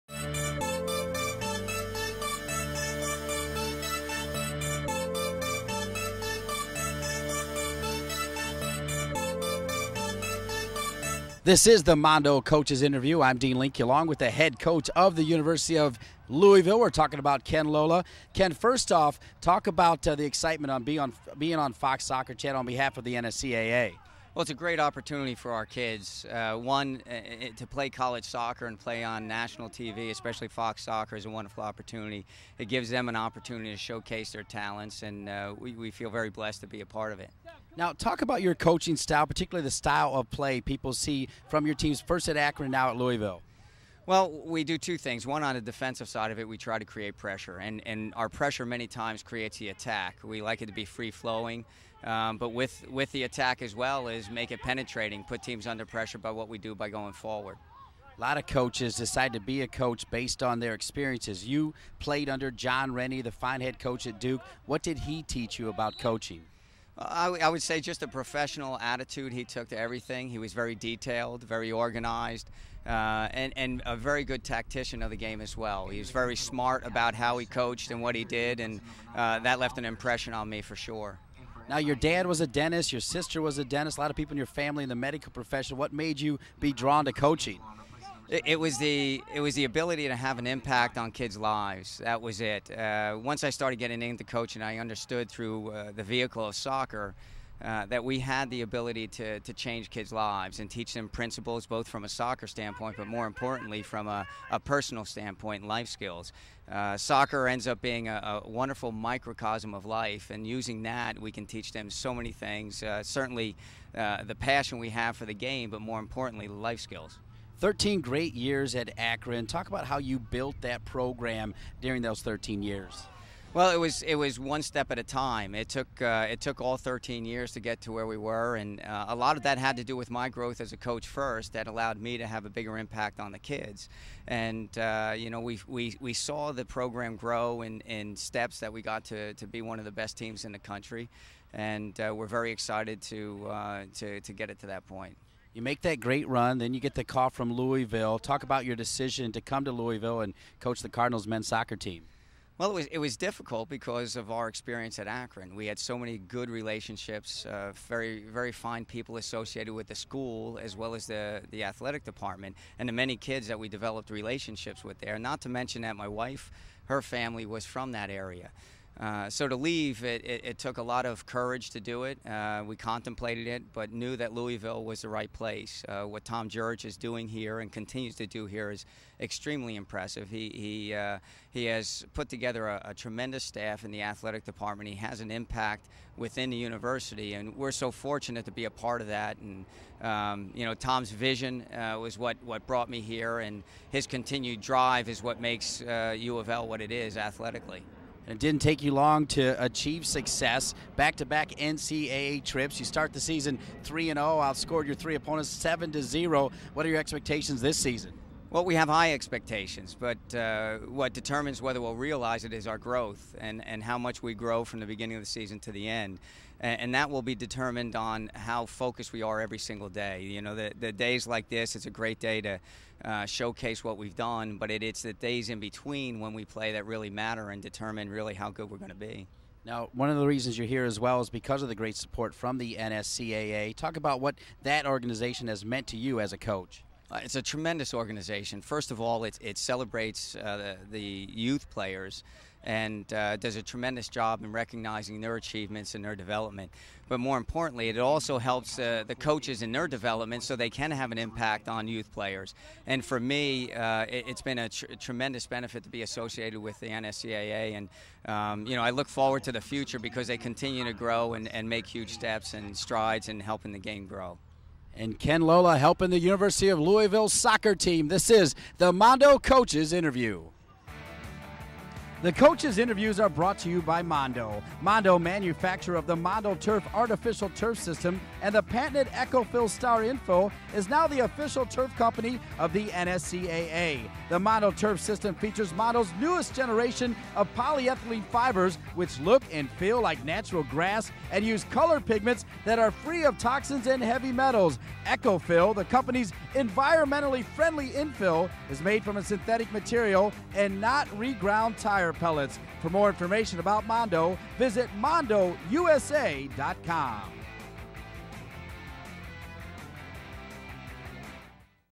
NSCAA College Soccer Game of the Week Coaches Interview - Ken Lolla
Throughout the 2009 season, Mondo USA - The Official Turf Sponsor and Supplier of the NSCAA and NSCAA Convention - is proud to bring you exclusive interviews with NSCAA College Game of the Week coaches.  Featured here is University of Louisville men's coach Ken Lolla, prior to his squad's 1-1 draw with UIC on September 13, 2009.